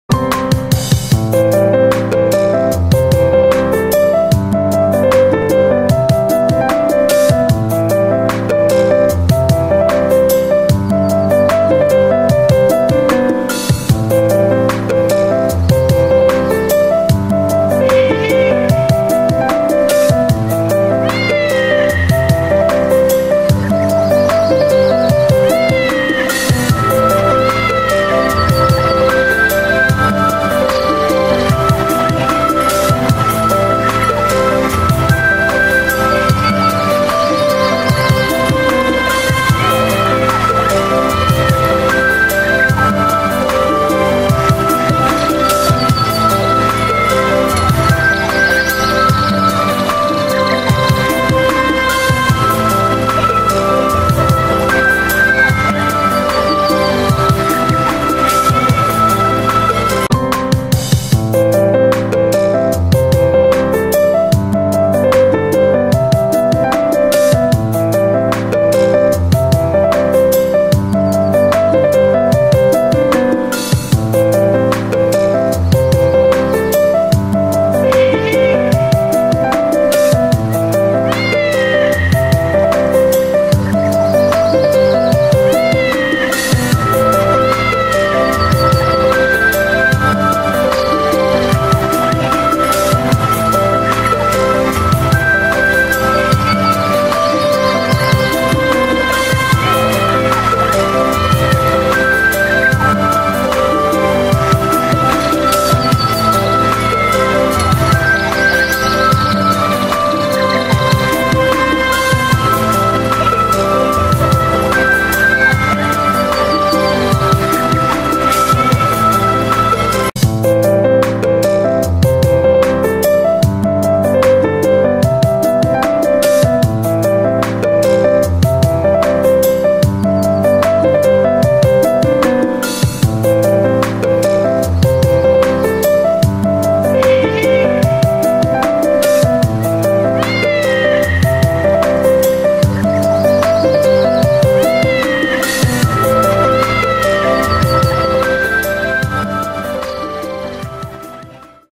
ZT6090 UV Flatbed printer for uv film
big color uv flatbed printer with 3 pcs high quality printhead. it can print color+white+varnish at same time. Max printing height 250mm. Also it can printing bottle and cups.
you could order to print metal, tile, glass, acrylic and so on.